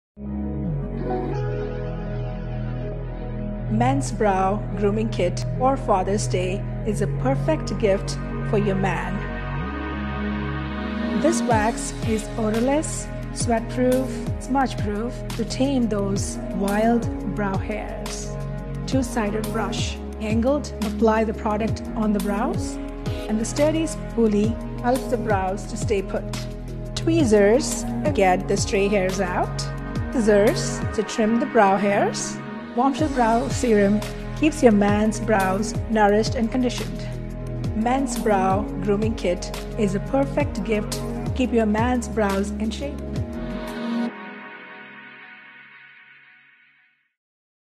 Father’s Day with Reema’s Limited Edition Men’s Brow Grooming Kit
⭐️ Celebrate the special man  in your life  this Father’s Day with Reema’s Limited Edition Men’s Brow Grooming Kit.   BROWJOB Men’s Grooming Kit by Rahel has everything  he’ll need  to get his brows in shape plus a a handy travel case and includes the following:⠀⠀⠀⠀⠀⠀⠀⠀⠀
• Badass Brow Wax⠀⠀⠀⠀⠀⠀⠀⠀⠀
• Bombshell Brow Serum⠀⠀⠀⠀⠀⠀⠀⠀⠀
• Golden Tweezers⠀⠀⠀⠀⠀⠀⠀⠀⠀
• Golden Scissors⠀⠀⠀⠀⠀⠀⠀⠀⠀
• Black Mesh Signature Bag⠀⠀⠀⠀⠀⠀⠀⠀⠀